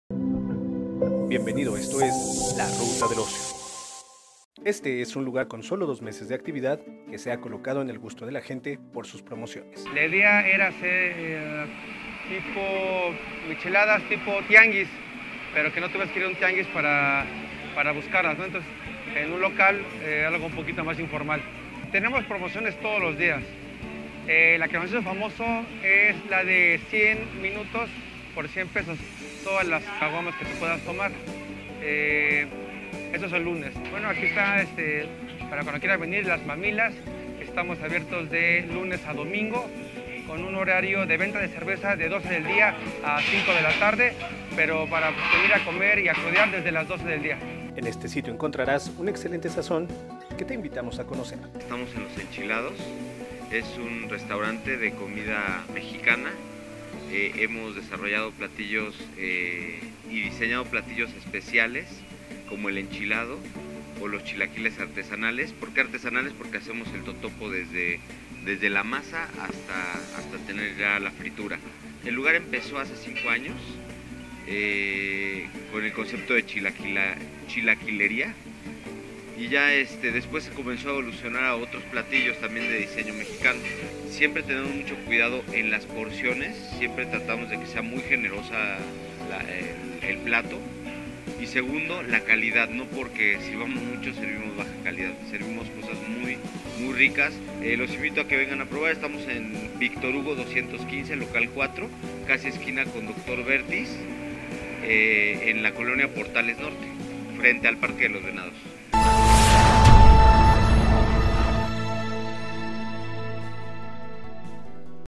La Ruta del Ocio | Parque de los Venados
Las Mamilas es un negocio donde puedes disfrutar de una buena michelada y ricos platillos de mar, aquí encontrarás todos los días promociones como todas las caguamas que puedas tomar en 100 minutos, por 100 pesos.
Por último "Los Enchilados", es un restaurante de comida mexicana con un exquisito sazón, aquí las porciones en el platillo son muy generosas y con una gran calidad, visítalos.